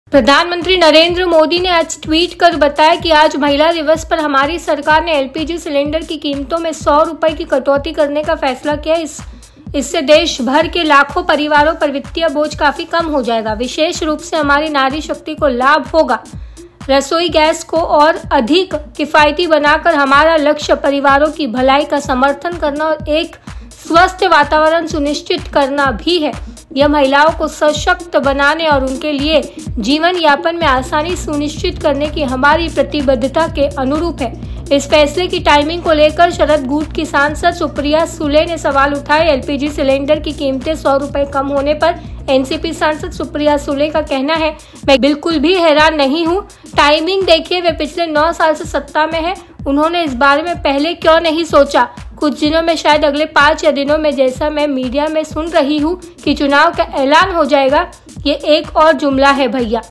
Supriya Sule ने पीएम मोदी पर साधा निशाना, एलपीजी रसोई गैस की कीमतों में कटौती के फैसले को बताया जुमल
प्रधानमंत्री नरेंद्र मोदी ने आज ट्वीट कर बताया कि "आज महिला दिवस पर, हमारी सरकार ने एलपीजी सिलेंडर की कीमतों में 100 रुपये की कटौती करने का फैसला किया है. इससे देश भर के लाखों परिवारों पर वित्तीय बोझ काफी कम हो जाएगा, विशेष रूप से हमारी नारी शक्ति को लाभ होगा. रसोई गैस को और अधिक किफायती बनाकर, हमारा लक्ष्य परिवारों की भलाई का समर्थन करना और एक स्वस्थ वातावरण सुनिश्चित करना भी है. यह महिलाओं को सशक्त बनाने और उनके लिए 'जीवनयापन में आसानी' सुनिश्चित करने की हमारी प्रतिबद्धता के अनुरूप है."
इस फैसले की टाइमिंग को लेकर शरद गुट की सांसद सुप्रिया सुले ने सवाल उठाए हैं. एलपीजी सिलेंडर की कीमतें 100 रुपये कम होने पर एनसीपी (एससीपी) सांसद सुप्रिया सुले का कहना है, 'मैं बिल्कुल भी हैरान नहीं हूं. टाइमिंग देखिए. वे पिछले 9 साल से सत्ता में हैं. उन्होंने इस बारे में पहले क्यों नहीं सोचा? कुछ दिनों में शायद अगले 5 या 6 दिनों में जैसा मैं मीडिया में सुन रही हूं कि चुनाव का एलान हो जाएगा. ये एक और जुमला है भैया. ये सब राजनीति है ये दिल से नहीं किया गया है. हमारी सरकार में सिलेंडर 430 रुपये था. वे इसकी बराबरी क्यों नहीं करते?'